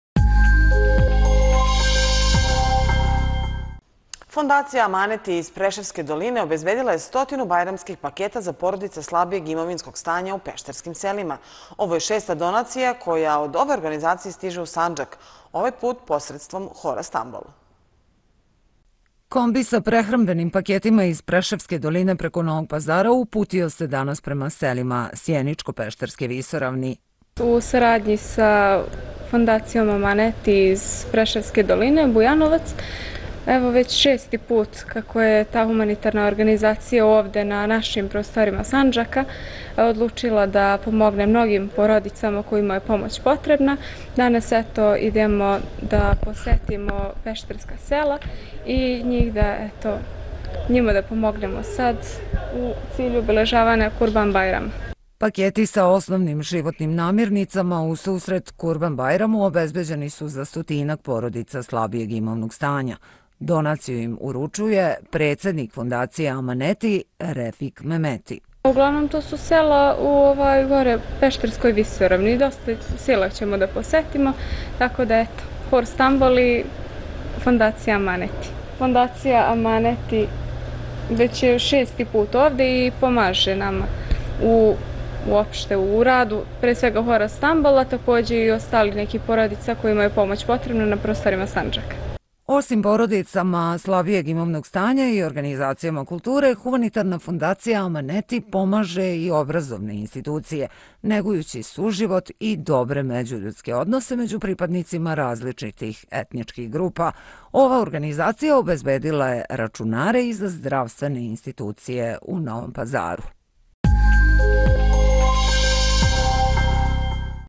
Bajramski paketi iz Preševske doline do pešterskih sela
Fondacija ''Amaneti'' iz Preševske doline obezbedila je 100 bajramskih paketa za porodice slabijeg imovnog stanja u pešterskim selima. Ovo je šesta donacija koja od ove organizacije stiže u Sandžak, ovaj put posredstvom hora ''Stambol''.